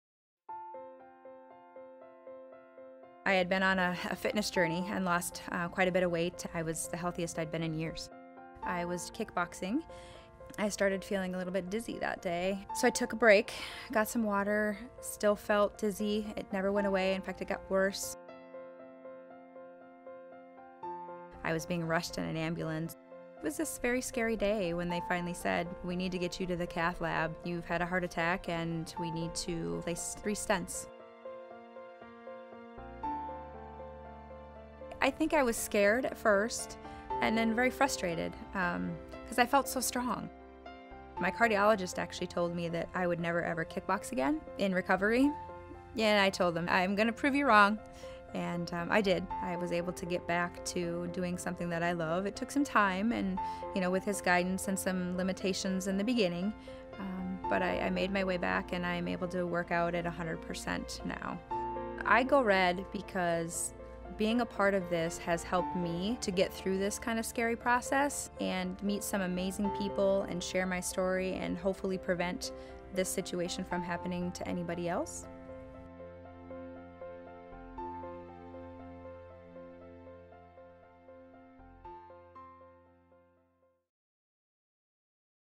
Stephanie Lang - National Go Red For Women Spokesperson
Stephanie Lang, from Minnesota, was in the midst of her lifestyle change journey losing nearly 100 pounds when she suffered a SCAD (spontaneous coronary artery dissection -- where the inside of the artery tears creating a blockage) heart attack during a workout. She's sharing her story as a National Go Red For Women spokesperson.